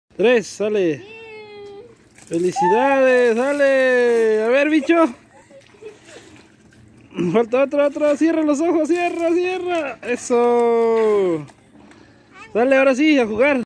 Así bañaron al champiñon (4 años)
Así felicitan a los niños en su cumpleaños